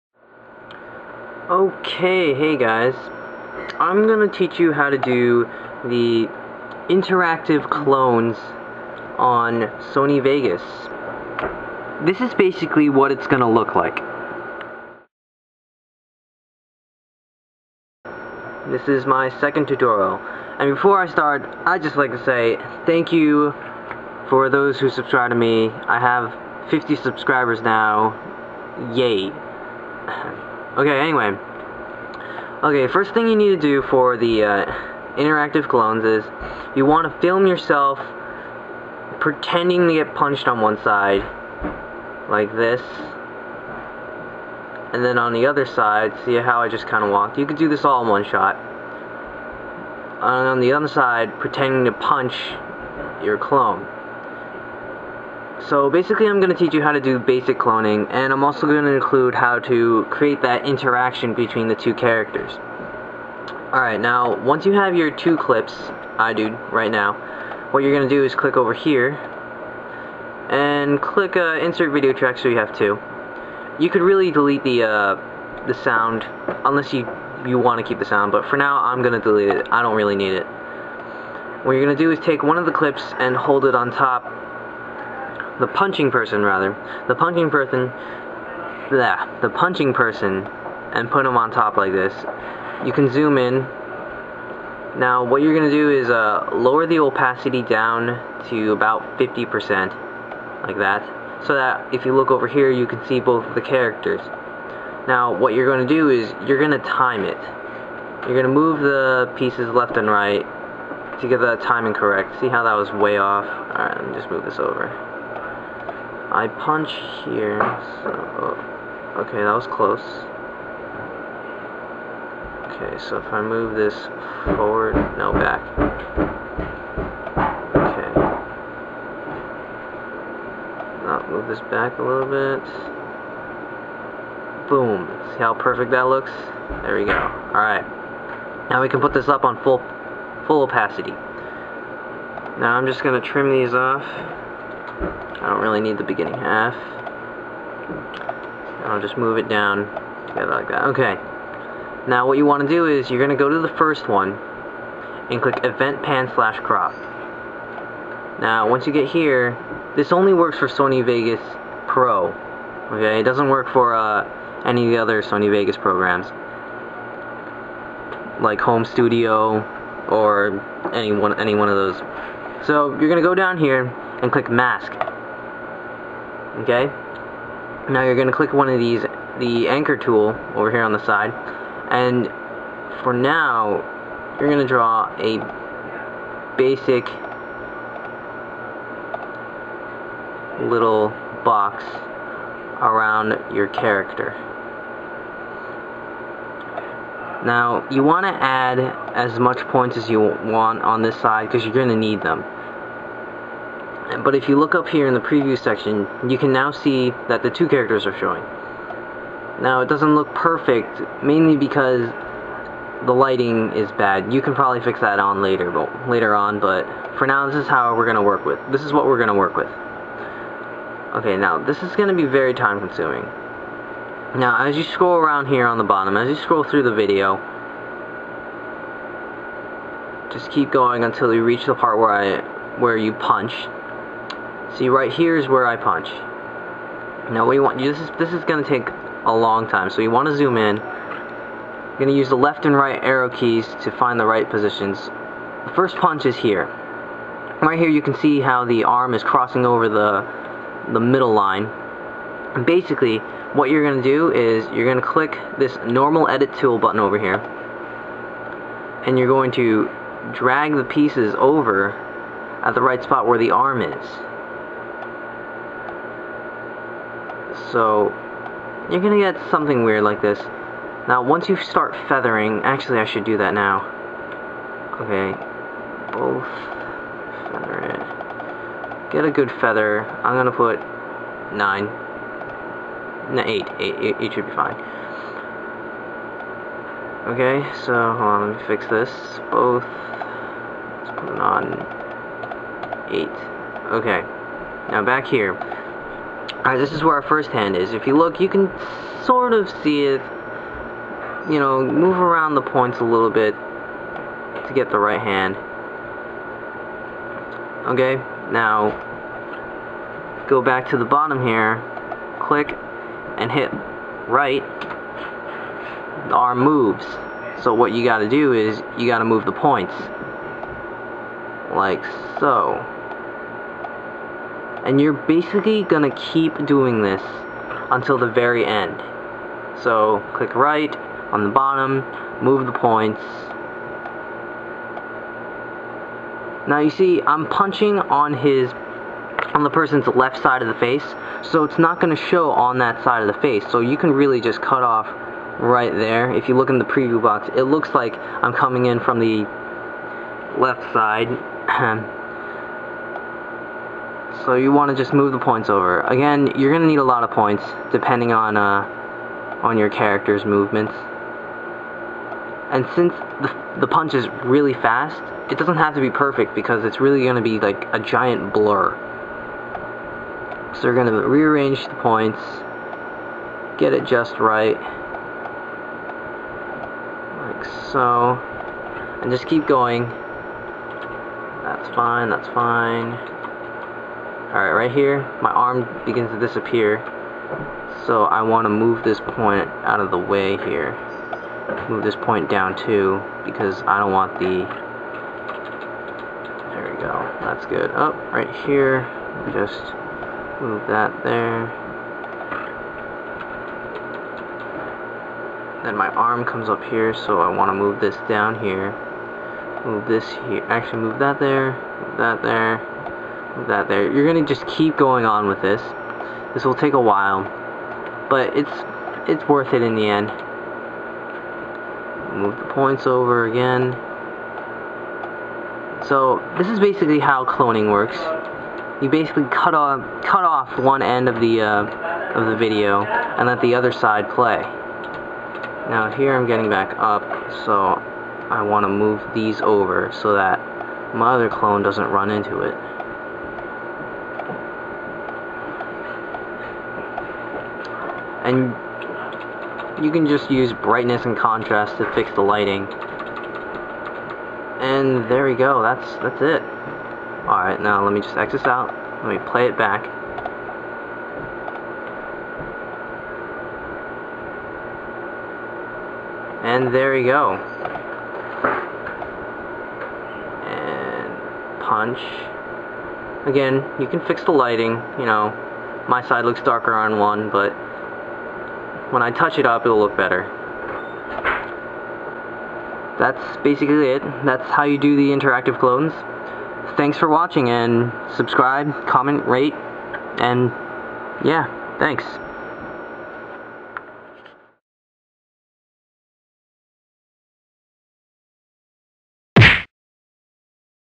Sony Vegas Punching your Clone Tutorial
Anyways, here's my 2nd tutorial on Sony Vegas Effects. This tutorial will go through cloning and making them interact with each other, for example, punching each other!

Sorry if the video seems rushed and the weird sound quality!